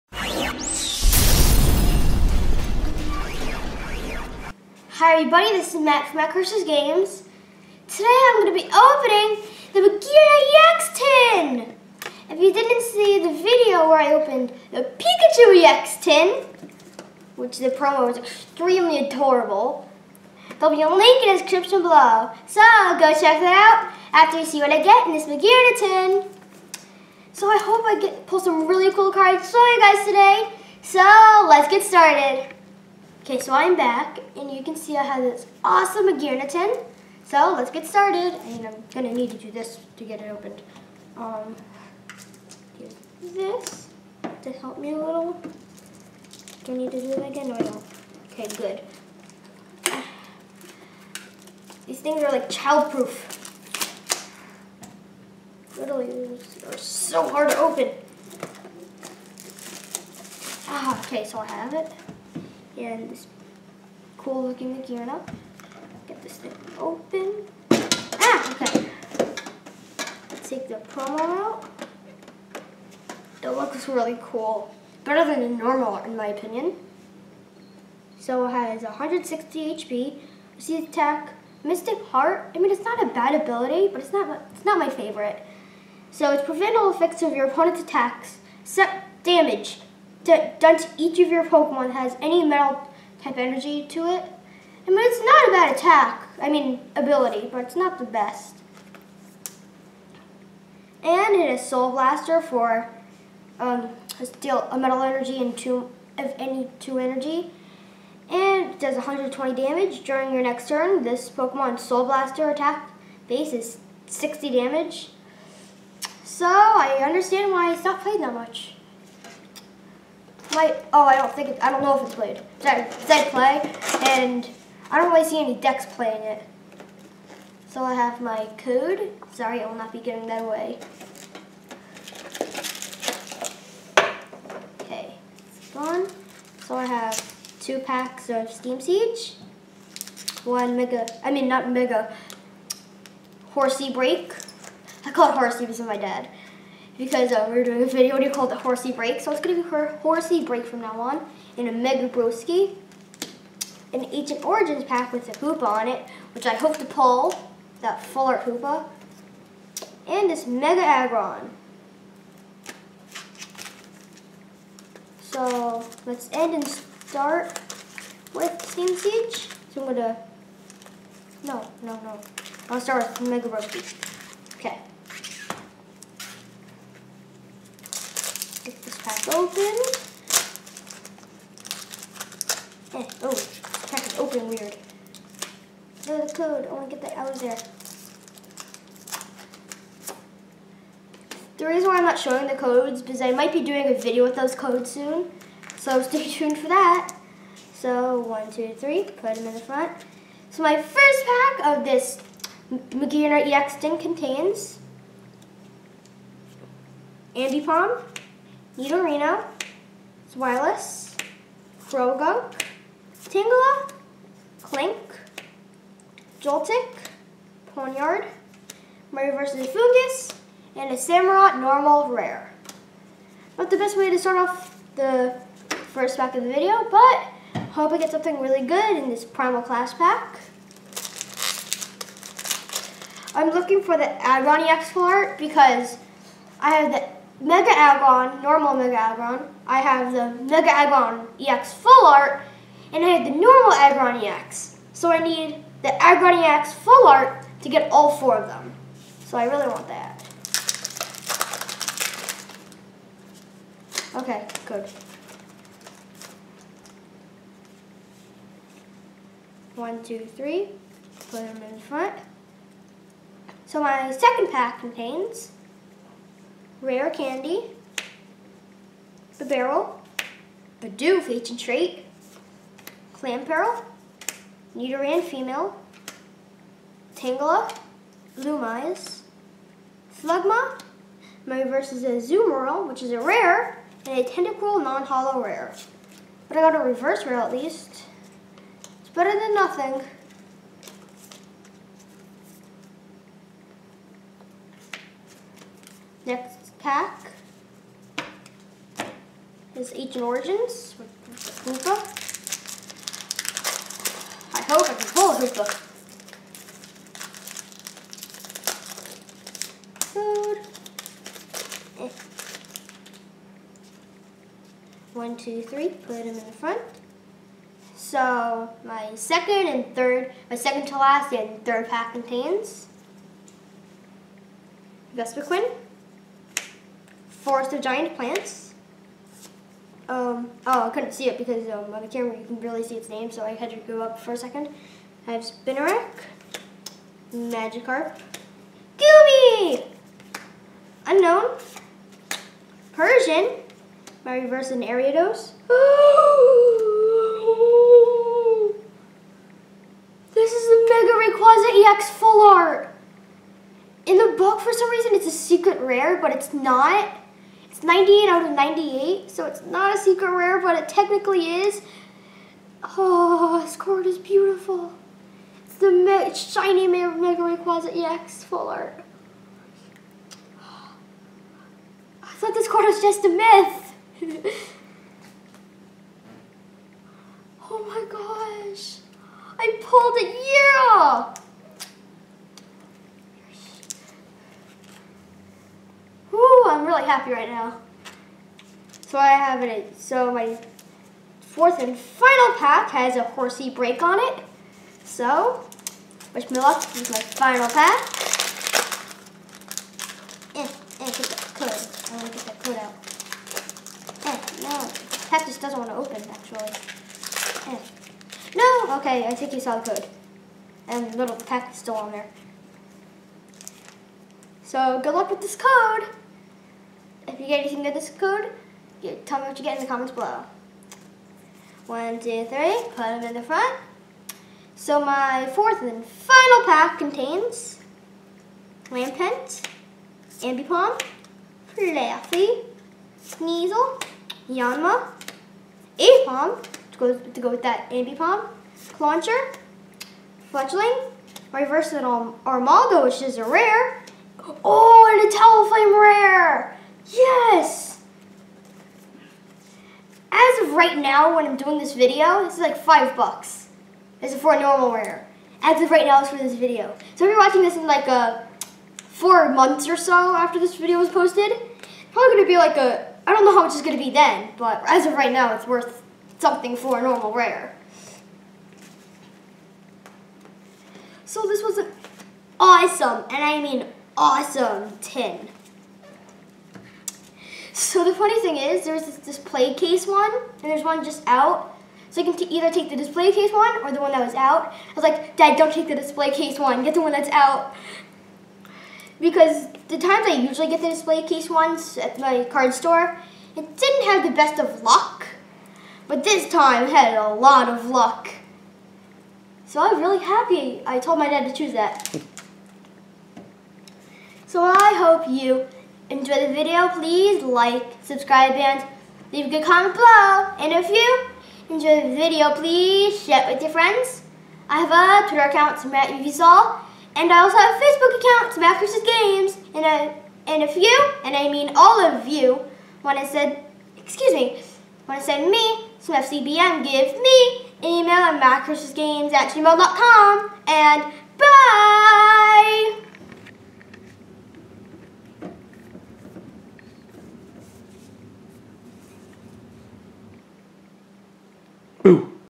Pokemon Magearna EX Battle Heart Tin Opening - I thought this SECRET RARE was a MYTH!
I saw the Magearna EX Battle Heart tin in the display case at my LGS, but they also had one on the shelf.  I took the one on the shelf ... and WOW am I glad I did!  I pulled a Secret Rare that I thought was a MYTH! You have to check out what pulls I got!

Watch until the very end for a SURPRISE!